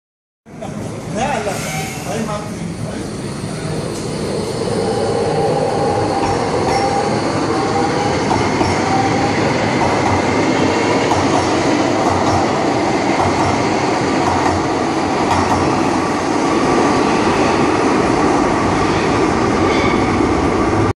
RET Metro 2*Type SG3 vertrekt op (verkeerd spoor) Oostplein naar Hoogvliet
RET Metro Type R (SG3) rijtuigen: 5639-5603 vertrekt op andere spoor van Oostplein op lijn C tot Hoogvliet.

Materieel: Tweewagentrein SG3
Station: Oostplein
Lijn: C tot Hoogvliet
WD: 554
Tijd: 21:18
Data: Vrijdag 07 november 2014